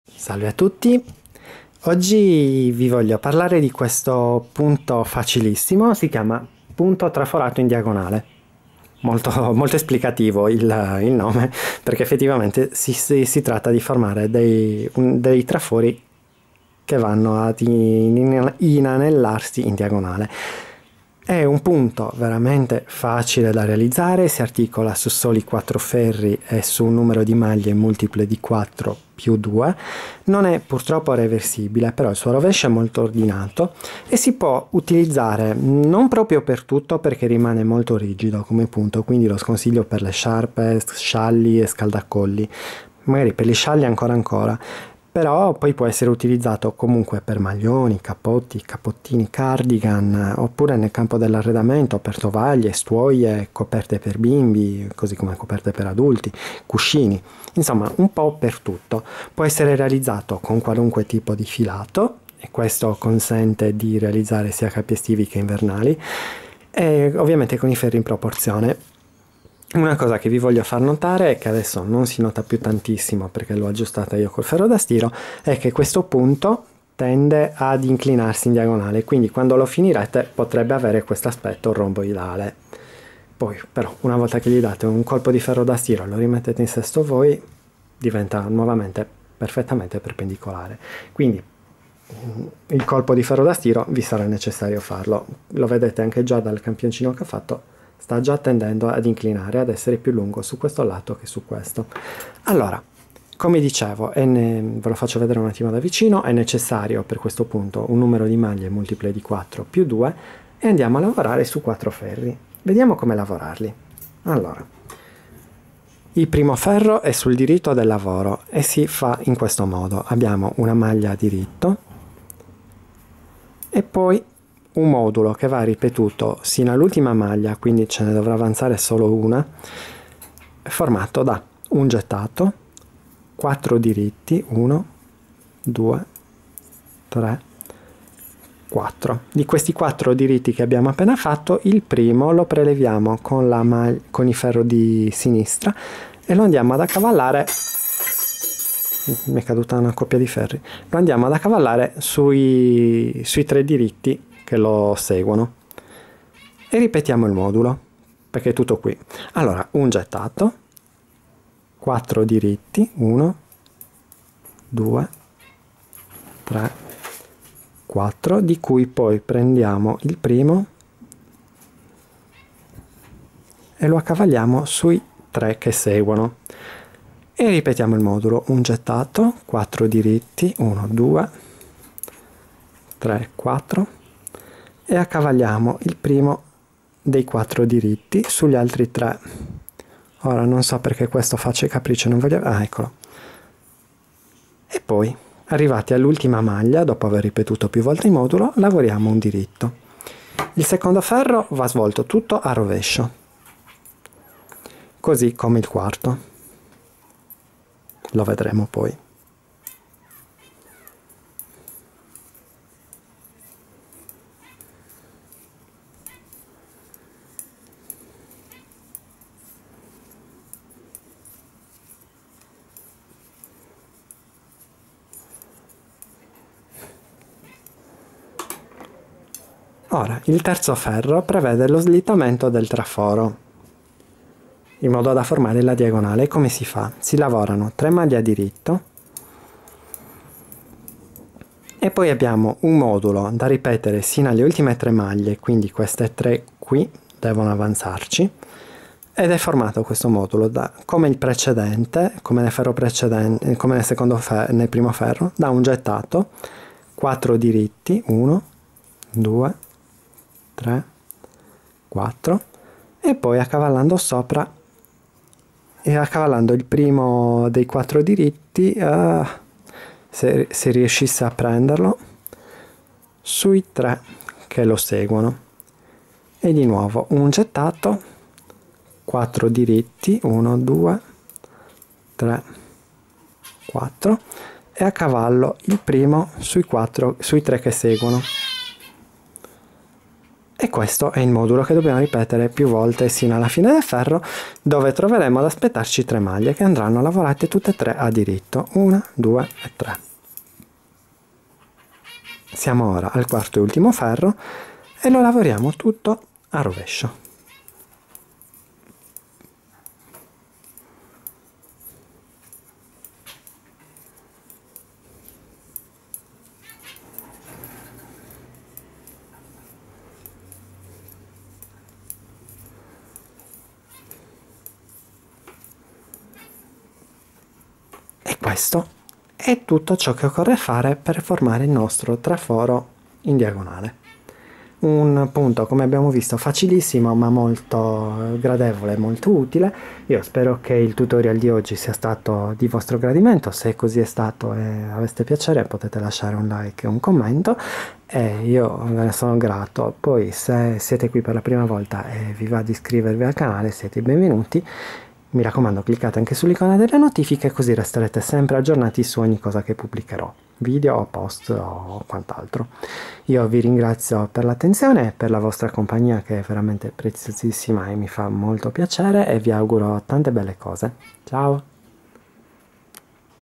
Traforo Diagonale a maglia
Cliccate qui 👇 per lo schema e le istruzioni - Click here for instructions and pattern 👇 :
Un punto semplicissimo e di bell'aspetto. Può essere utilizzato sia per l'abbigliamento (soprattutto per i capi invernali) che per l'arredamento. Si lavora bene con ogni tipo di filato.